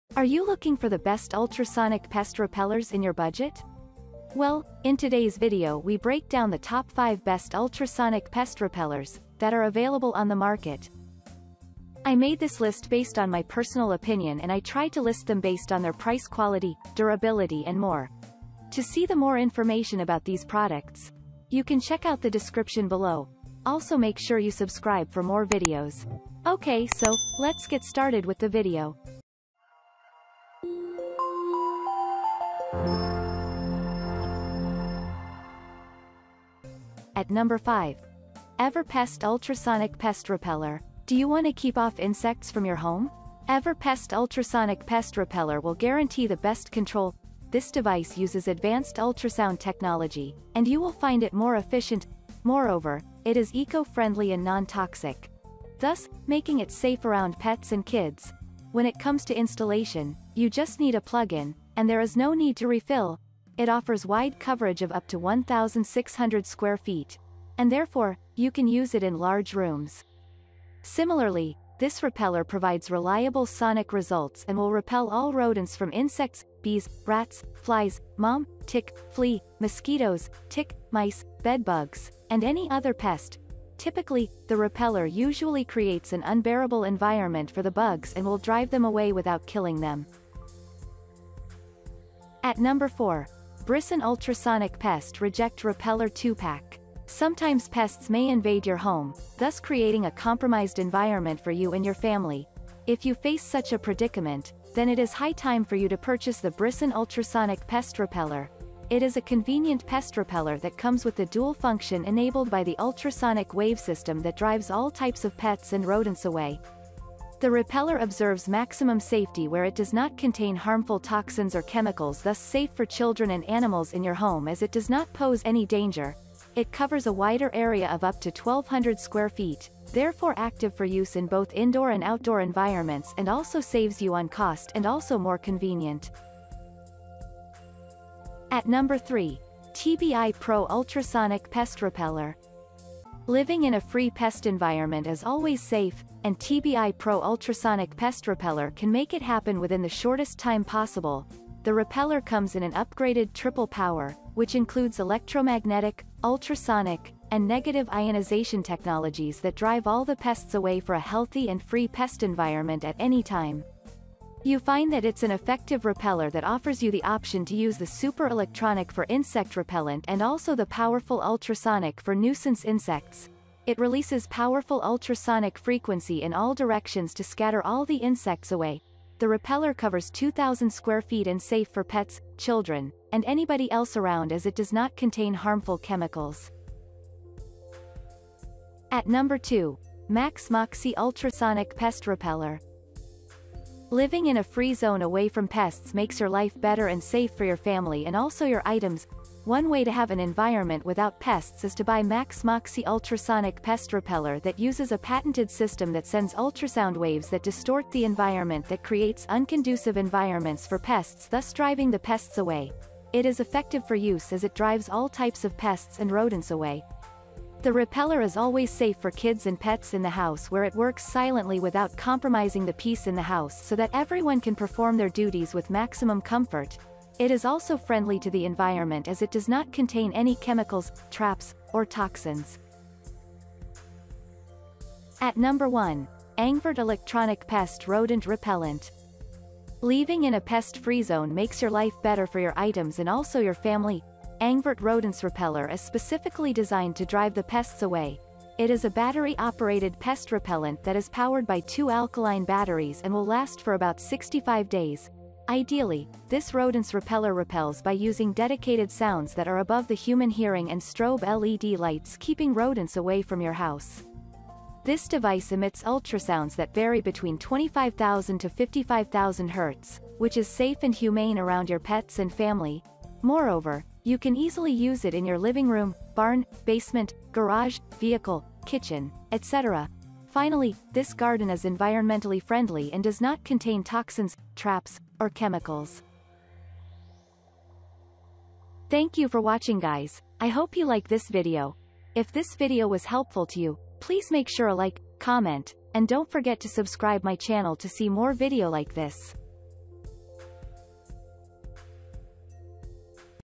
Top 5 Best Ultrasonic Pest Repellers in 2022
☛ All The Links to Best Ultrasonic Pest Repellers listed in this Video:-

► 5.Ever Pest Ultrasonic Pest Repeller.

► 4. BRISON Ultrasonic Pest Reject Repeller 2 pack.

► 3. TBI Pro Ultrasonic Pest Repeller.

► 2.MaxMoxie Ultrasonic Pest Repeller.

► 1.Angveirt Electronic Pest Rodent Repellent.

In This Video, I listed the Best Ultrasonic Pest Repellers That are available in the market. I made this list based on my personal research, and I tried to list them based on their price, quality,  and many more things.